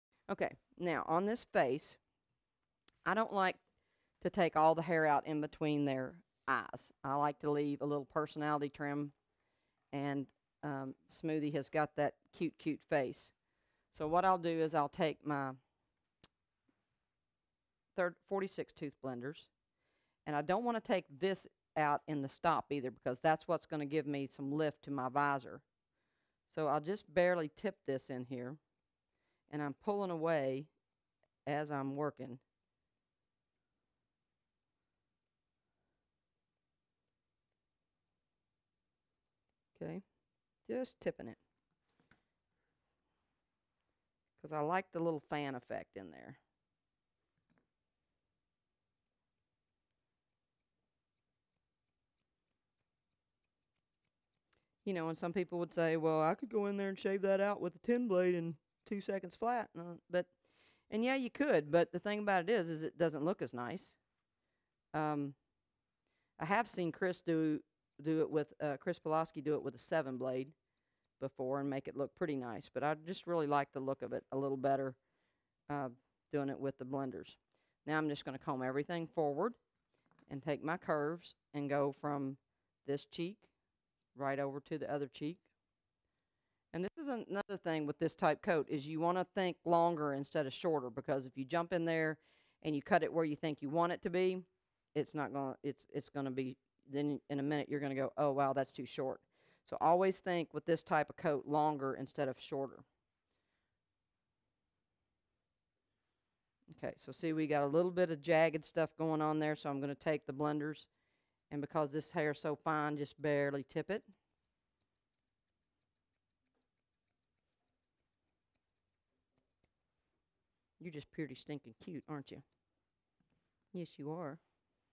Grooming a Silky Terrier Mix In a Cute Personality Trim
In less than 30 minutes, Judy turn this little rag-a-muffin looking little dog into something cute. This dog has one of those challenging coats to get smooth. It's soft. It's silky. It has no body. It marks and tracks like crazy! Judy makes the grooming look easy with the right tools and techniques. As a bonus, this is a young dog that is a bit fussy at times on the table. You get to see firsthand how Judy 'table trains' her dogs. As she says, "The least amount of restraint is the best restraint" as she works with this dog right on camera. By the end of the session, this little dog not only looks adorable for an everyday shop style trim -- but he has learned to trust and respect the grooming process.